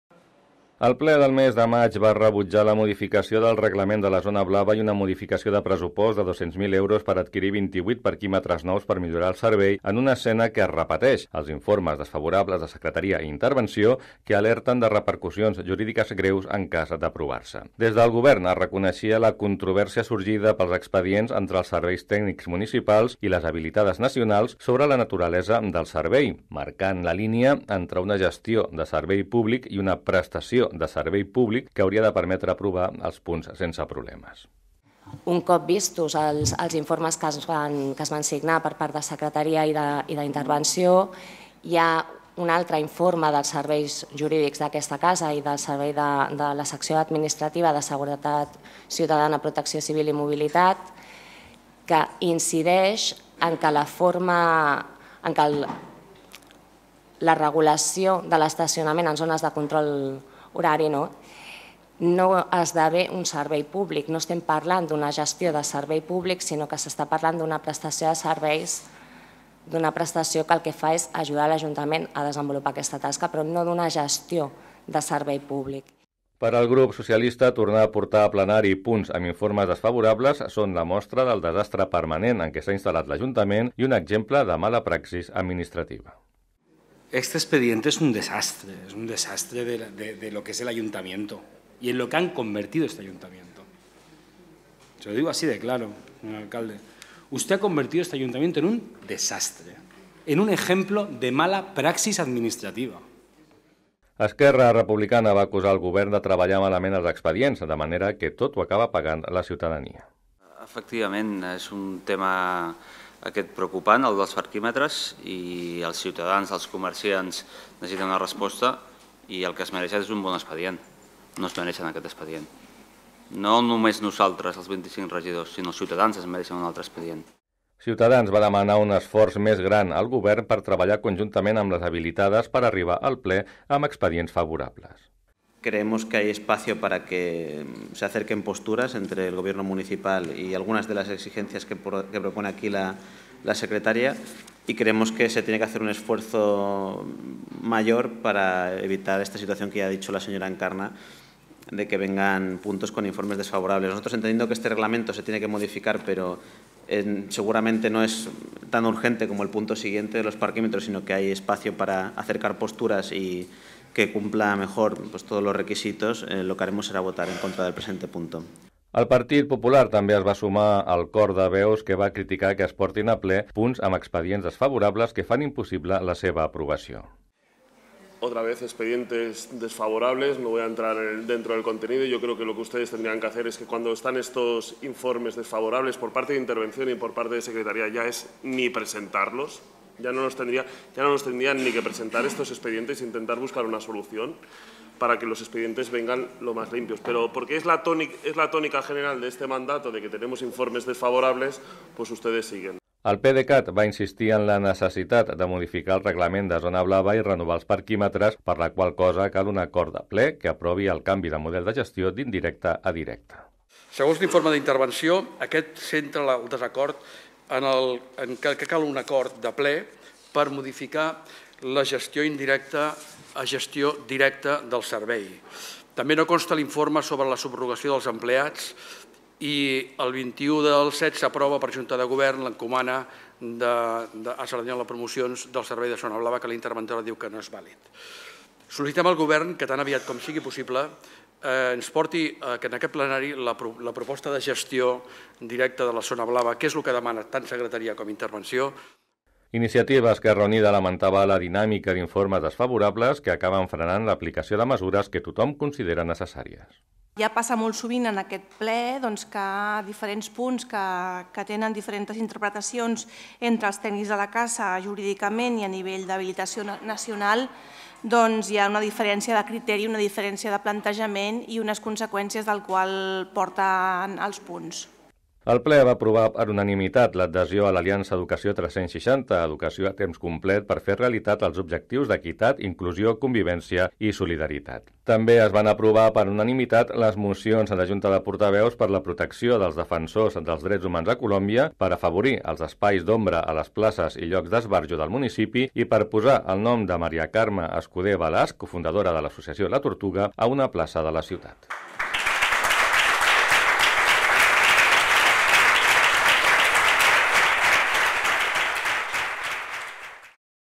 Ple municipal (maig de 2018)
Un nou confús debat al Ple amb informes jurídics contradictoris rebutja els canvis a la zona blava